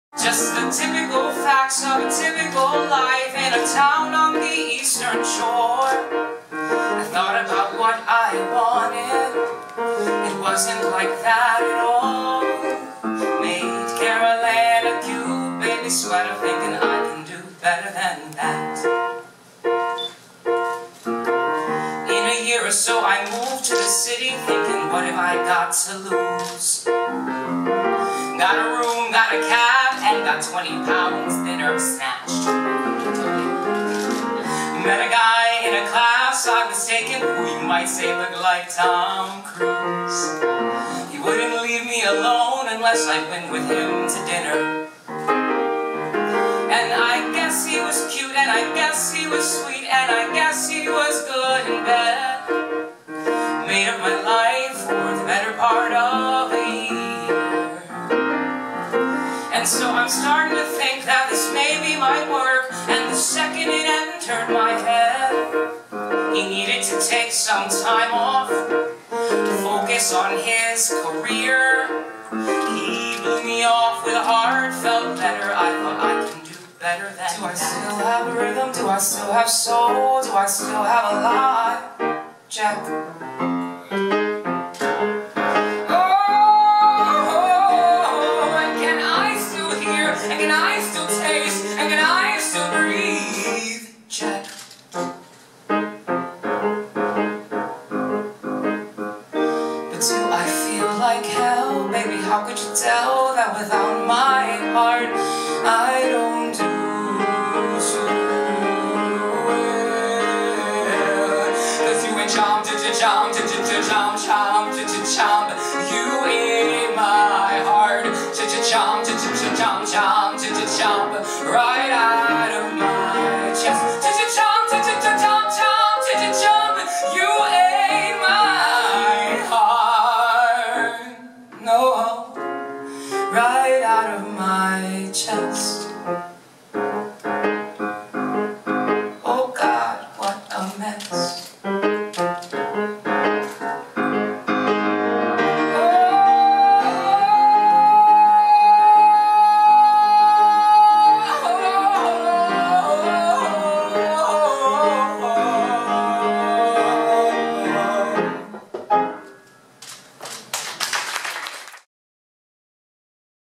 Robin DeJesus at 54 below
Robin DeJesus will be appearing at 54 Below in NYC on Nov 24, 25 2013